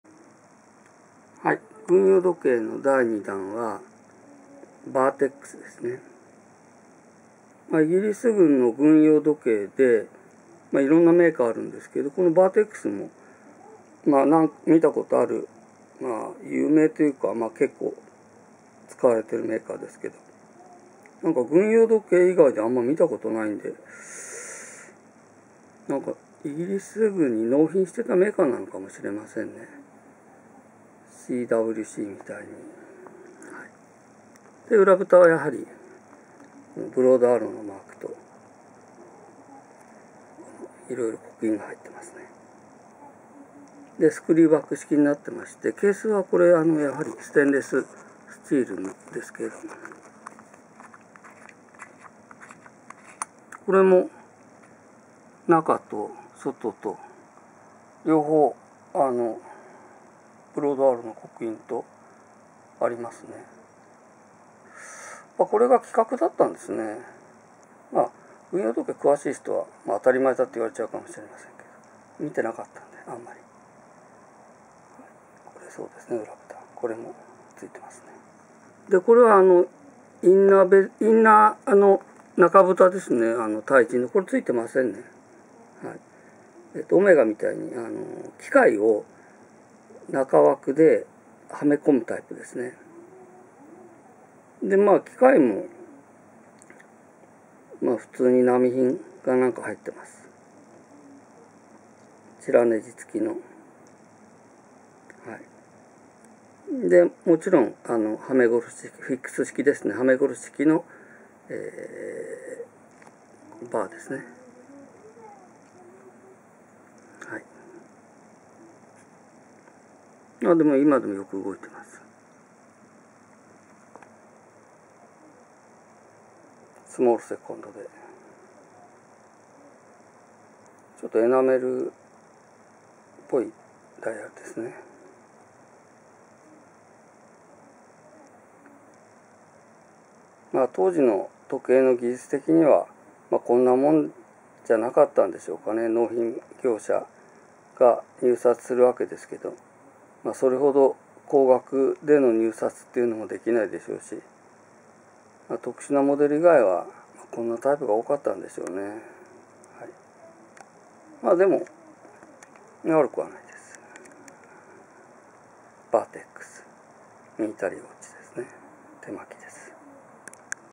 VERTEX MILITARY WATCH
VERTEX社の英国軍　ブロードアローの入った手巻きモデルです
ケースがSSステンレス製、機械は手巻き　CAL５９の１５石モデル
裏蓋には、W.W.W. ↑　A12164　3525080　刻印